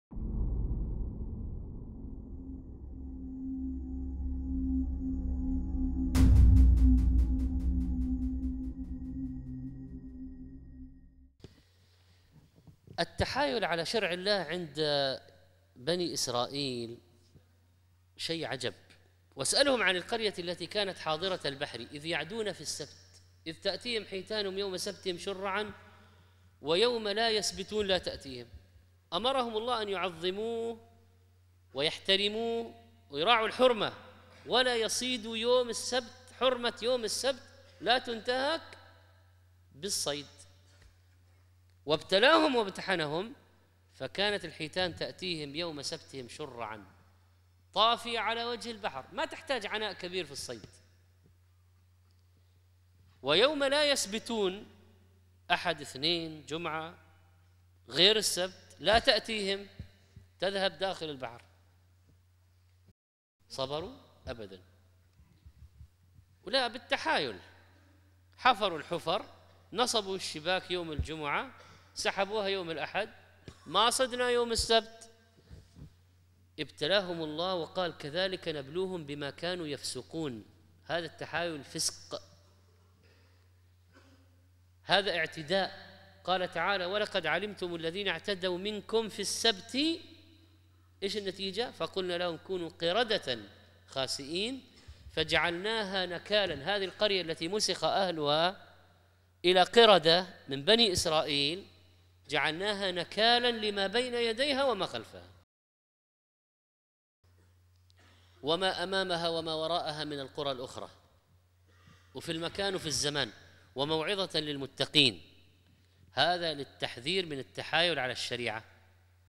تحايل بني إسرائيل على شرع الله / المنجد
تحايل بني إسرائيل على شرع الله
عنصر من محاضرة : أصحاب محمد وأصحاب موسى
للشيخ / محمد صالح المنجد